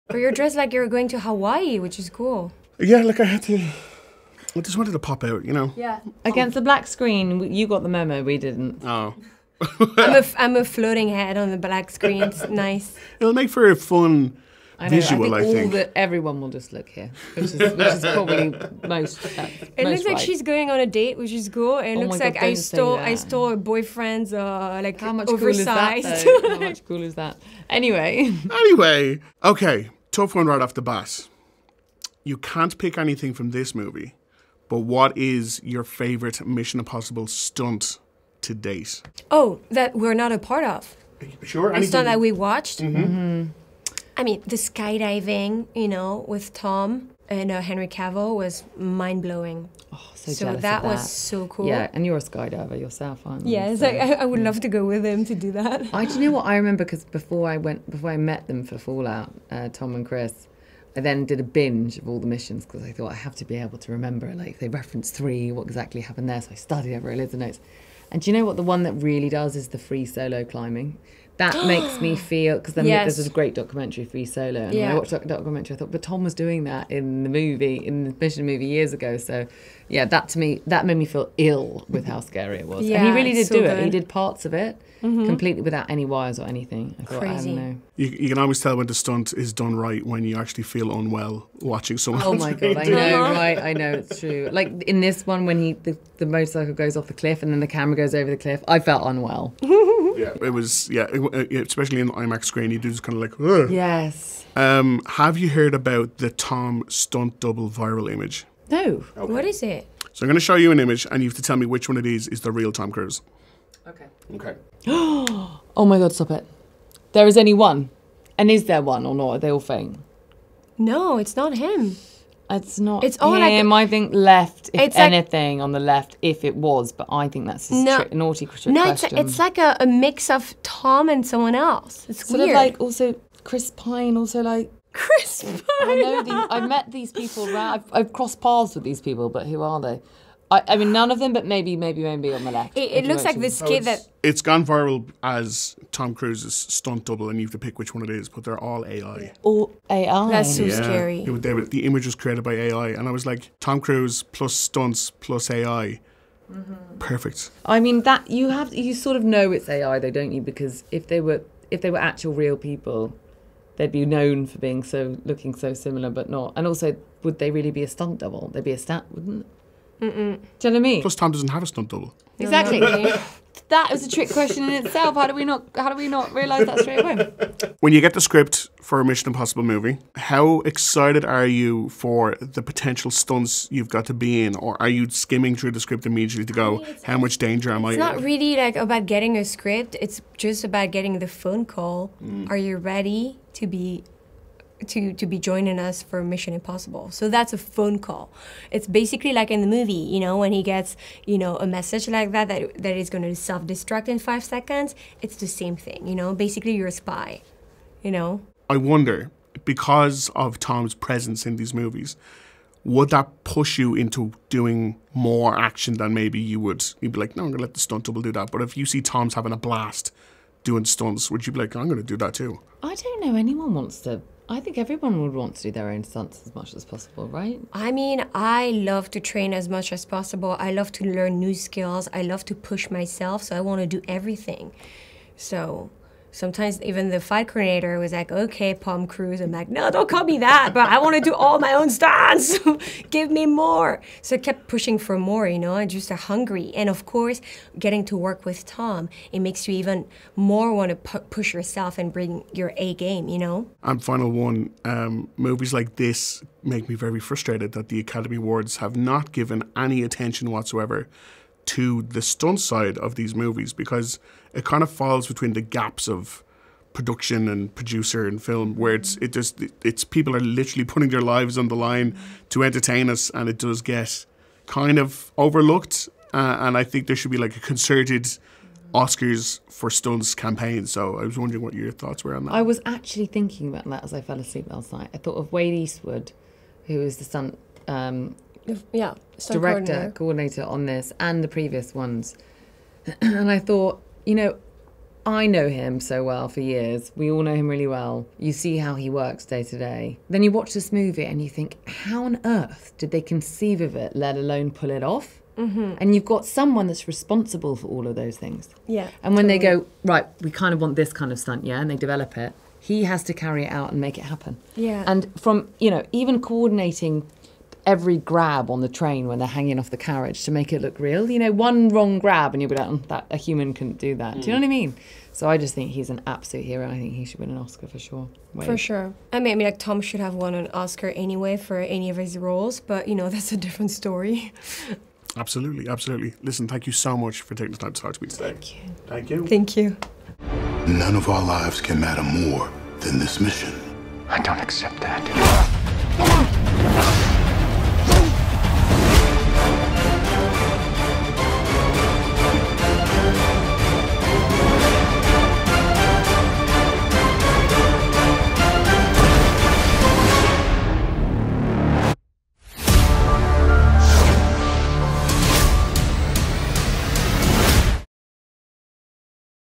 Pom Klementieff & Vanessa Kirby on the Mission Impossible stunt that made them physically unwell
We chatted to Pom Klementieff and Vanessa Kirby about their roles in huge new blockbuster sequel Mission: Impossible - Dead Reckoning Part One.